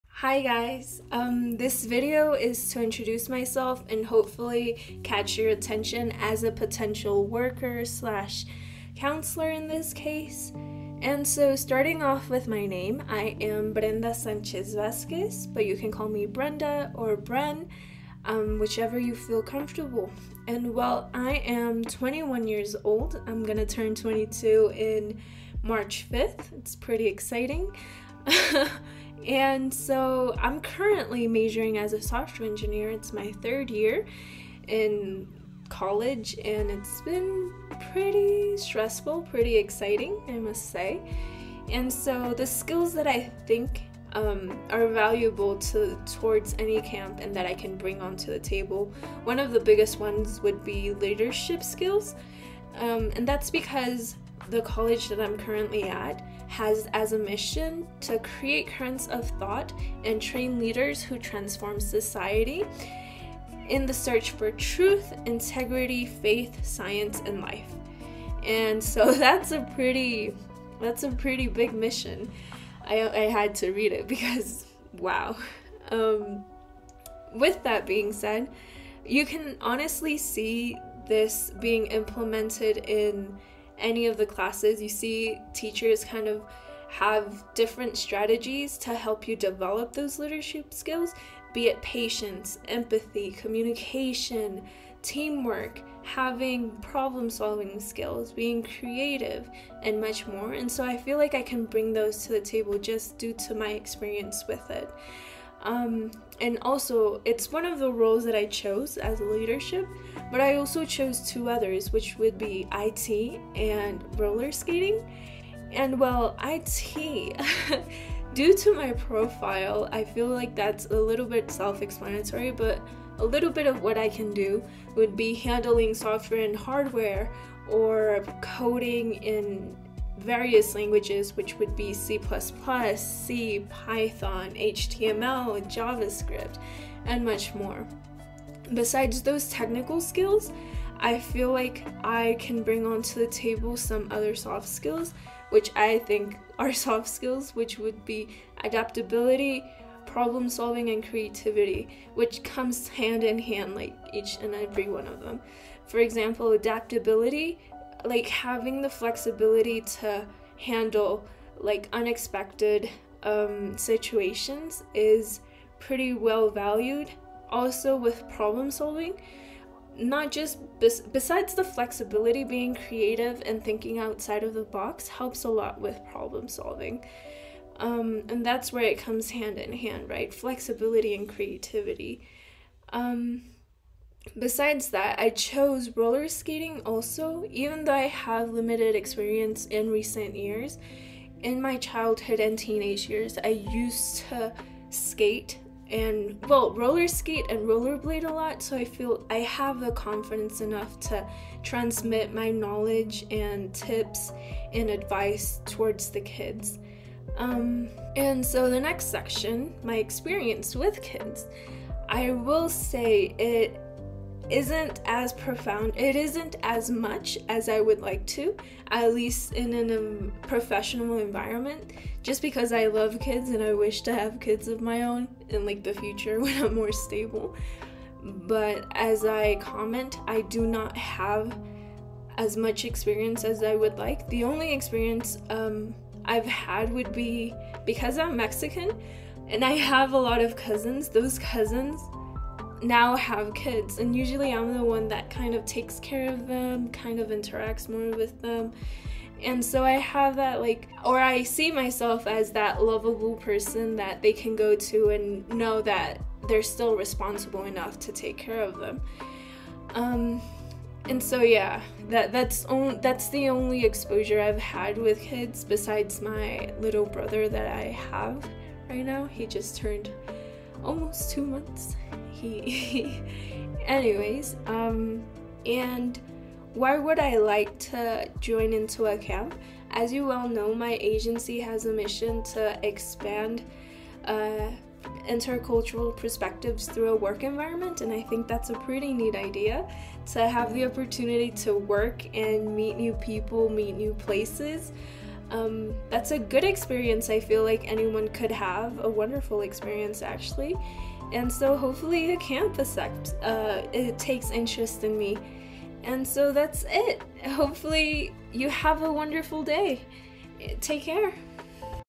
My Introduction Video for Summer Camp!
Hi!
I'm Brenda Sanchez Vazquez, in this video I showcase my skills and experience to hopefully get accepted at a camp!